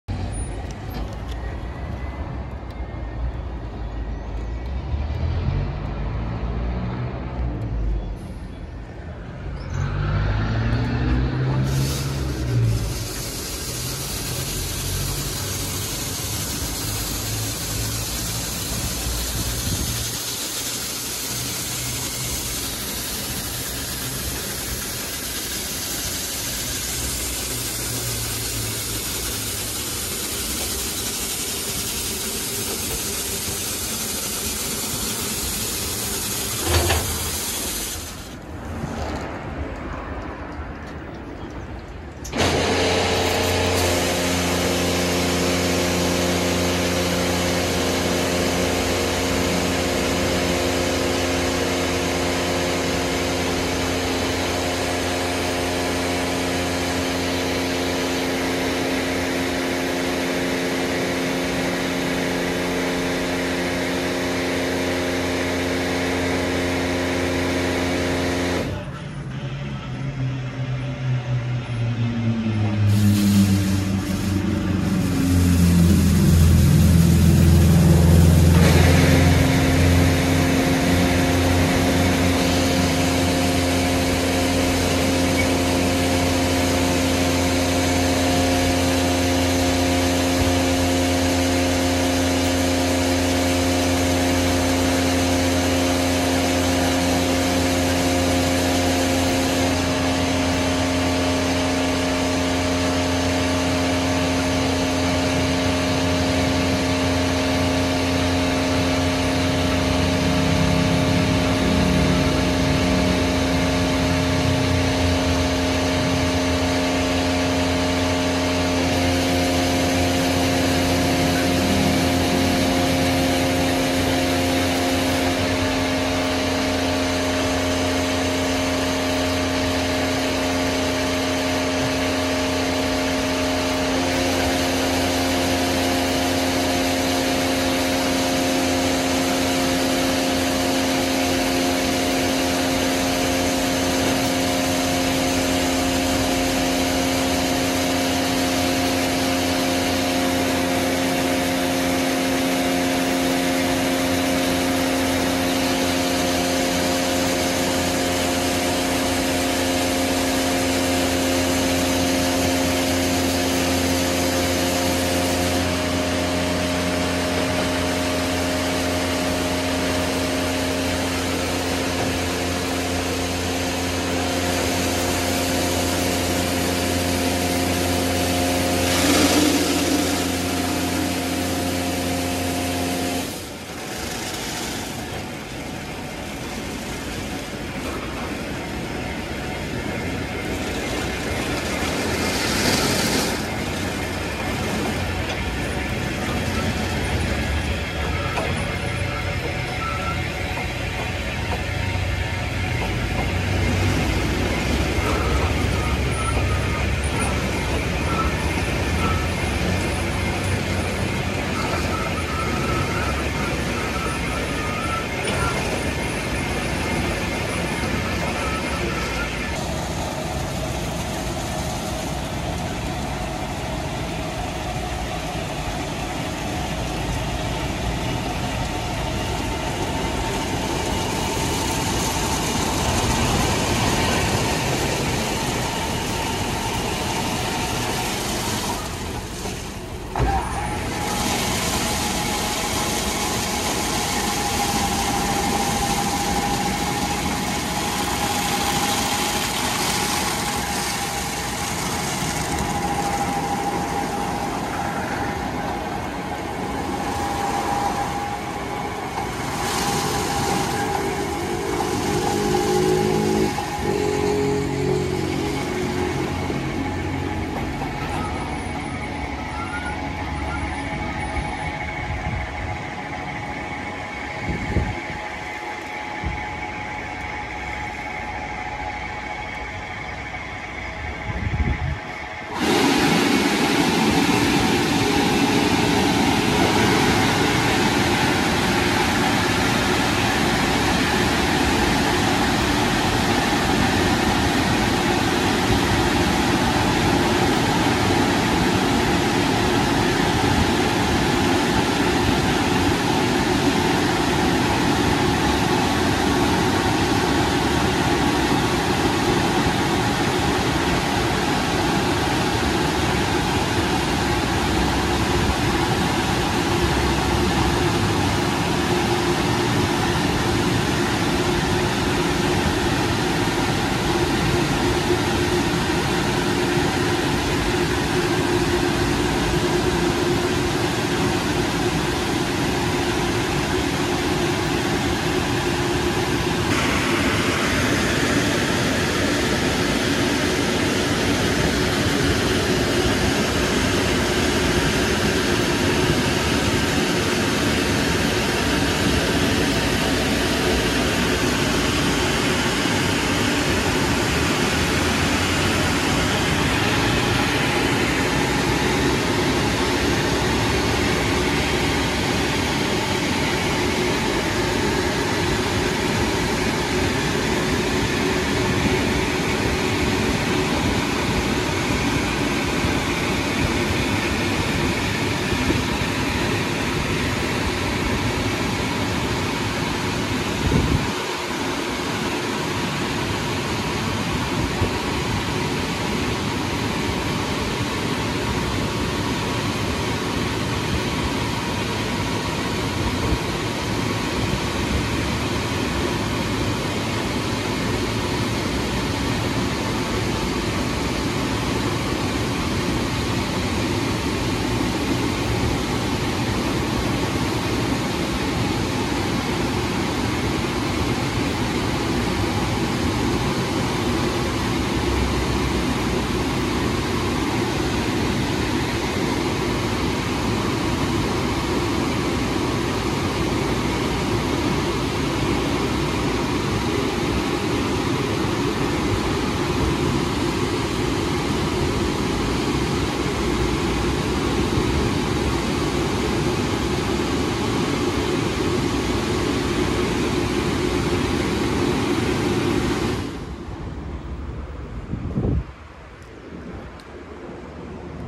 4K Istobal M1 New Brushes Revisit [Outside View]
This Istobal M1 was installed in 2020 and replaced a very worn Ceccato Winner. This car wash is always very busy and had the brushes replaced recently. The wash results were very good and consistent with all the cars that I watched wash but the only problem is that because of the high traffic it’s easy to have 5 to 6 cars waiting in line which means if you’re the last one you’ll wait 1 hour to get a car wash.

The wash includes:
- Active foam presoak
- Side high pressure
- Brushes with shampoo and wheel wash with high pressure
- Brushes rinse with wax
- 2x dryes